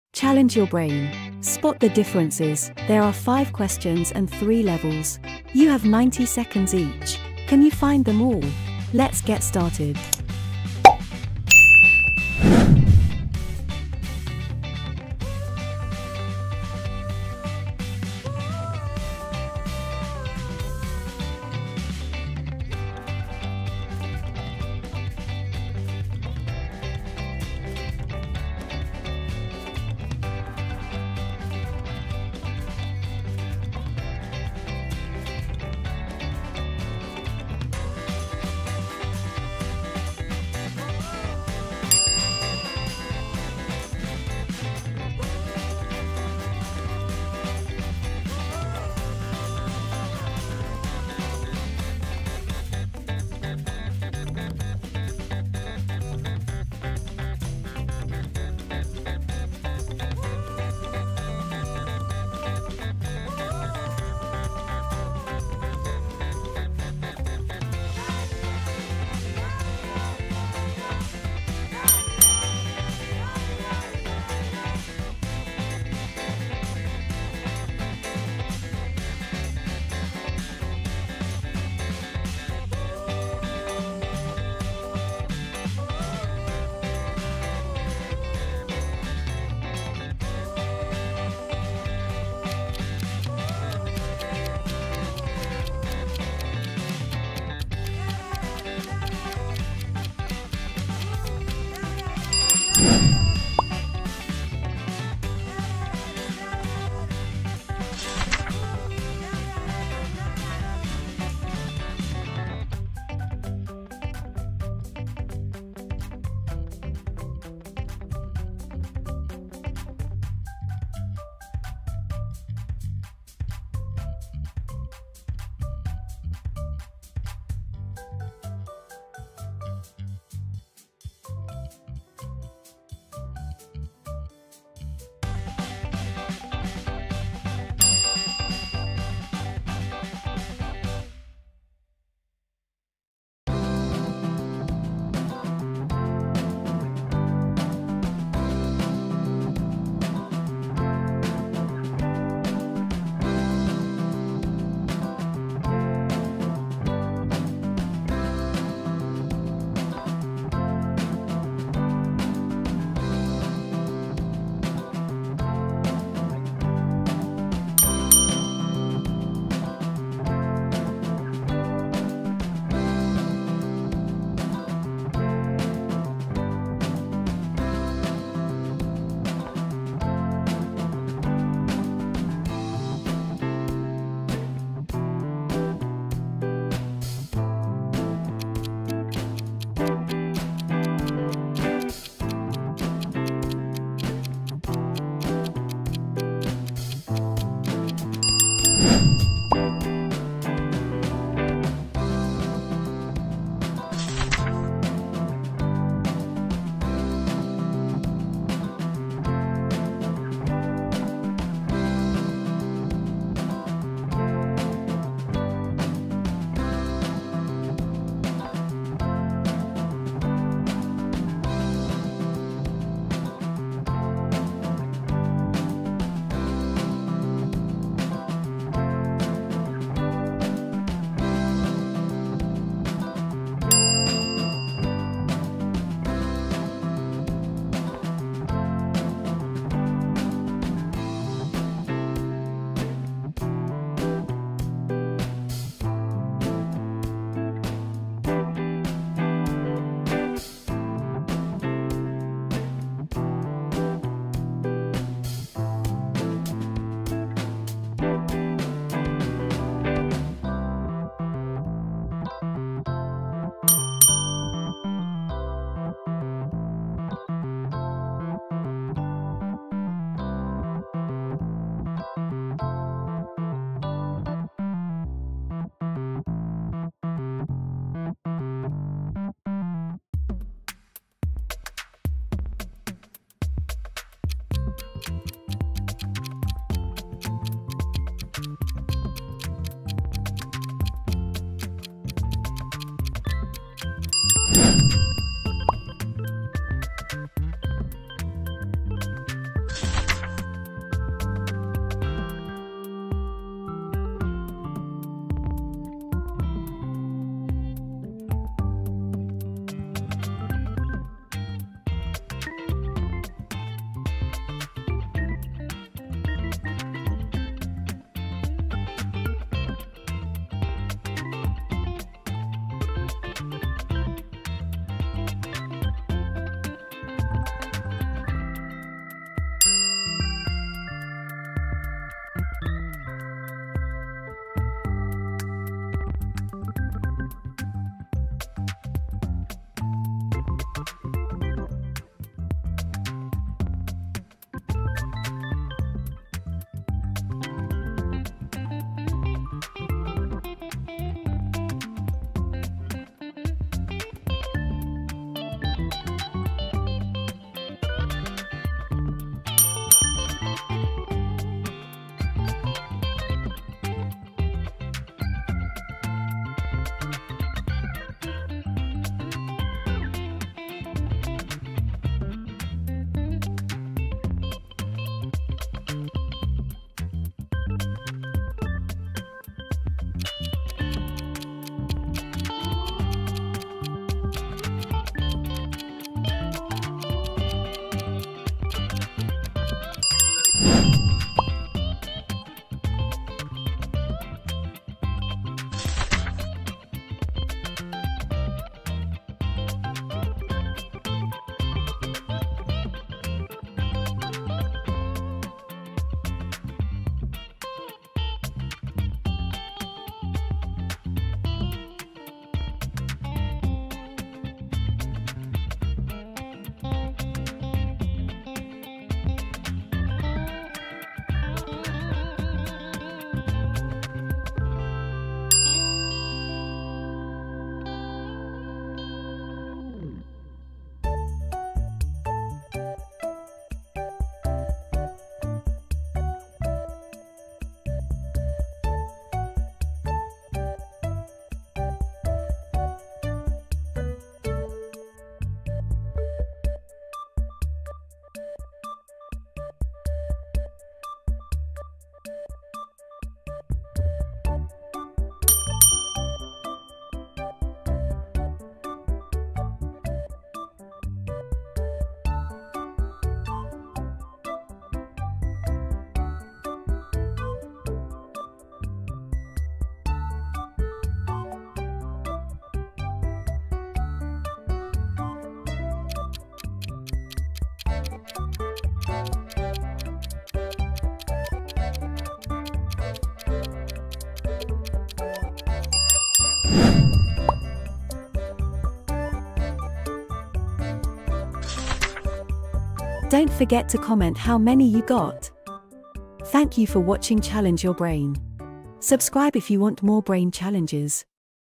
Spot the differences | Can you find them all? [Find the difference 36]
Can you spot the differences between the two pictures?

Welcome to Challenge your brain!
There are 5 questions and 3 levels - easy, medium and hard.
You have 90 seconds each.

Let us know how many you got!
Have Fun!